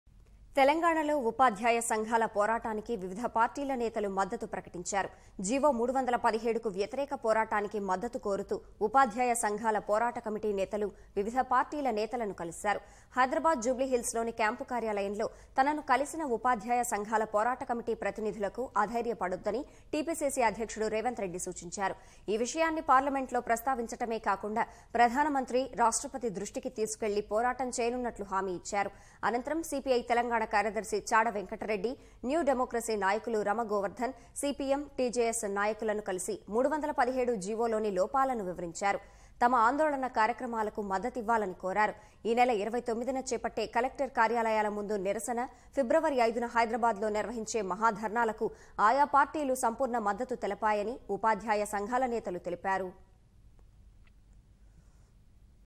రాష్ట్రంలో ఉపాధ్యాయుల పోరాటానికి పార్టీల మద్దతు | GO 317 | Leaders Provides Support to Teachers
తెలంగాణలో. ఉపాధ్యాయ సంఘాల పోరాటానికి వివిధ పార్టీల నేతలు మద్దతు ప్రకటించారు. G.O. 317కు వ్యతిరేక పోరాటానికి మద్దతు కోరుతూ. ఉపాధ్యాయ సంఘాల పోరాట కమిటీ నేతలు. వివిధ పార్టీల నేతలను కలిశారు. హైదరాబాద్ జూబ్లీహిల్స్ లోని క్యాంపు కార్యాలయంలో. తనను కలిసిన ఉపాధ్యాయ సంఘాల పోరాట కమిటీ ప్రతినిధులకు అధైర్య పడొద్దని. టీపీసీసీ అధ్యక్షుడు రేవంత్ రెడ్డి సూచించారు. ఈ విషయాన్ని పార్లమెంట్ లో ప్రస్తావించటమే కాకుండా. ప్రధానమంత్రి, రాష్ట్రపతి దృష్టికి తీసుకెళ్లి పోరాటం చేయనున్నట్లు హామీ ఇచ్చారు. అనంతరం సీపీఐ తెలంగాణ కార్యదర్శి చాడ వెంకటరెడ్డి, న్యూడెమోక్రసీ నాయకులు రమ గోవర్ధన్, C.P.M, T.J.S. నాయకులను కలిసి 317 G.O.లోని లోపాలను వివరించారు. తమ ఆందోళన కార్యక్రమాలకు మద్దతివ్వాలని కోరారు. ఈ నెల 29న చేపట్టే కలెక్టర్ కార్యాలయాల ముందు. నిరసన, ఫిబ్రవరి 5న హైదరాబాద్ లో నిర్వహించే మహాధర్నాలకు. ఆయా పార్టీలు సంపూర్ణ మద్దతు తెలిపాయని ఉపాధ్యాయ సంఘాల నేతలు తెలిపారు.